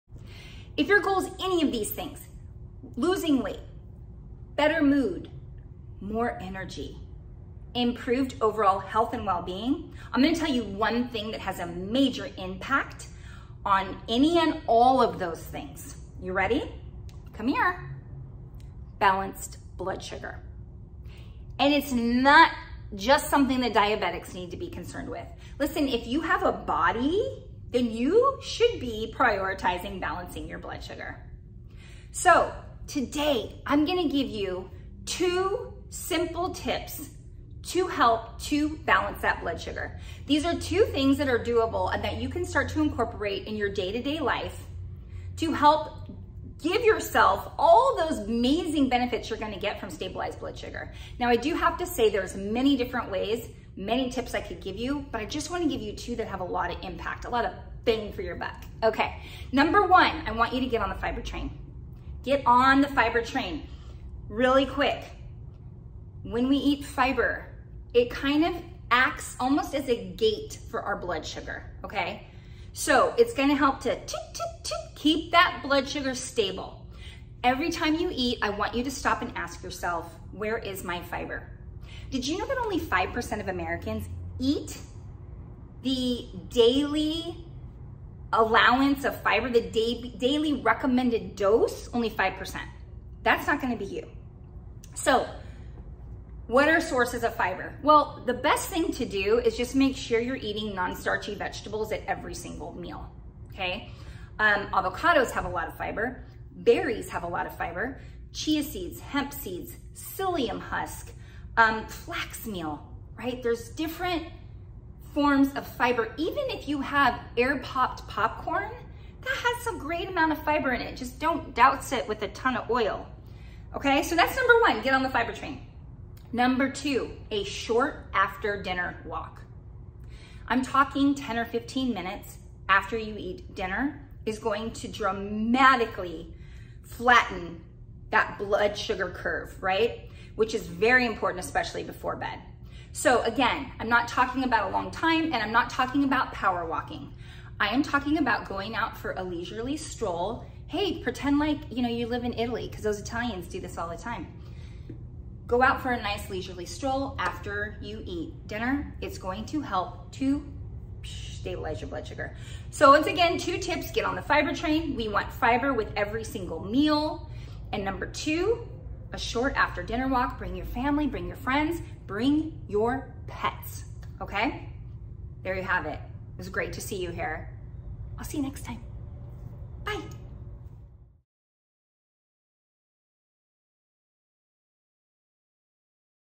ONE Thing with a MAJOR Impact on Weight + Overall Health
Do you want to lose weight? Have more energy? Improve your overall health and well-being?

This ONE thing has a HUGE impact on ALL of those things…

Let me tell you what it is and 2 Simple Ways to get more of it 😊